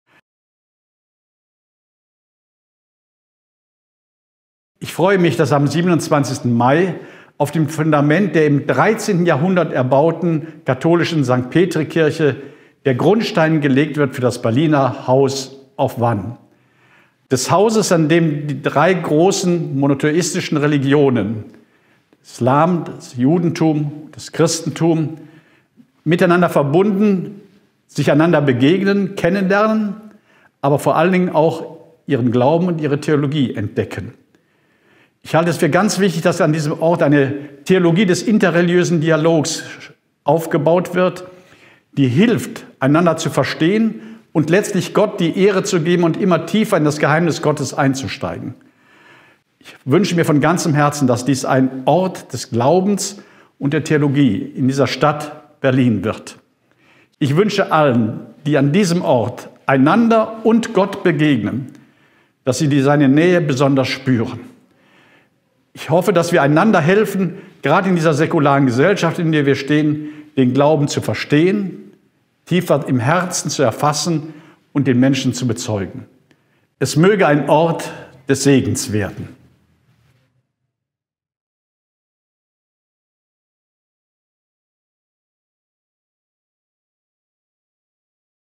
Lets Build Peace I Wishes for the House of One I Archbishop Dr. Heiner Koch
Wishes for the groundbreaking of the House of One from Archbishop Dr. Heiner Koch, Berlin/Germany:
"I think it is very important that in this place a theology of inter-religious dialogue will be built up which helps to understand each other."

Wünsche zur Grundsteinlegung von Erzbischof Dr. Heiner Koch, Berlin:
"Ich halte es für wichtig, dass an diesem Ort eine Theologie des interreligiösen Dialogs aufgebaut wird, die hilft einander zu verstehen."